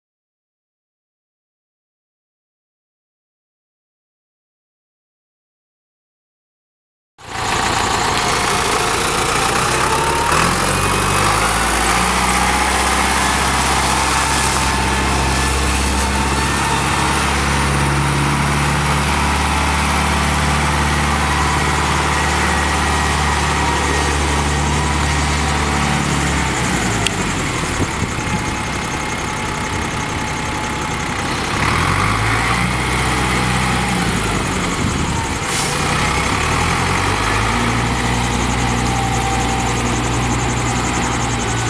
1998 Ford New Holland 6640SL tractor for sale | sold at auction October 27, 2015
1998 Ford New Holland 6640SL tractor
5,406 hours on meter
Four cylinder diesel engine
Powershift transmission
12F-12R gears
AC and heat
AM/FM
Three point
Top link
540
rear hydraulic remote sets
11-16 front tires
18.4-30 rear tires
93"W front axle
100"W rear axle
Battery dead
Left hood hinge will not open from the outside
Serial 087506B
Purchaser is responsible for all aspects of removal including adjusting axle width for transport.